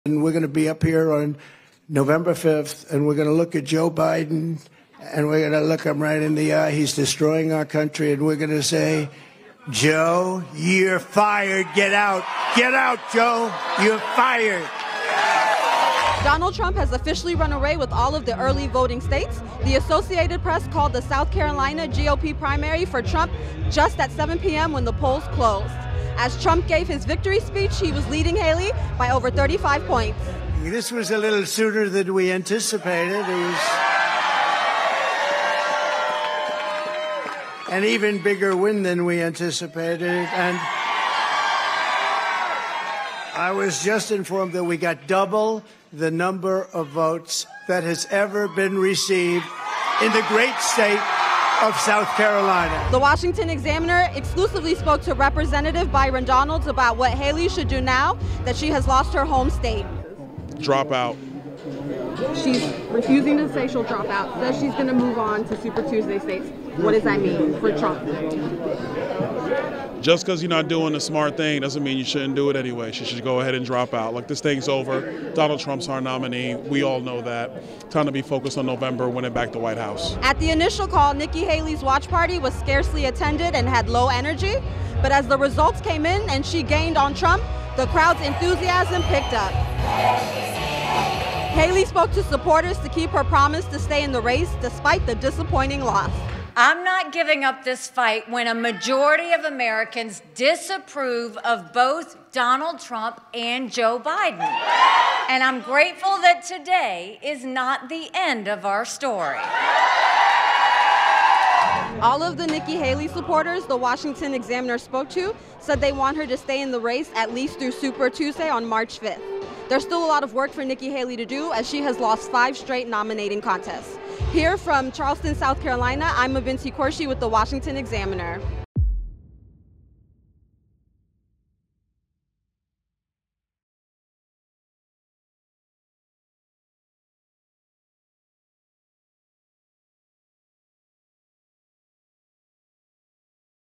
Nikki Haley refuses to drop out after South Carolina loss: ‘Today is not the end of our story’
CHARLESTON, South Carolina — Former U.S. Ambassador to the United Nations Nikki Haley said she will not give up her fight to be president after a crushing defeat to former President Donald Trump in her home state Saturday.

“I said earlier this week that no matter what happens in South Carolina, I would continue to run for president. I’m a woman of my word,” Haley said Saturday to cheers at her election night party at The Charleston Place. “I’m not giving up this fight when a majority of Americans disapprove of both Donald Trump and Joe Biden.”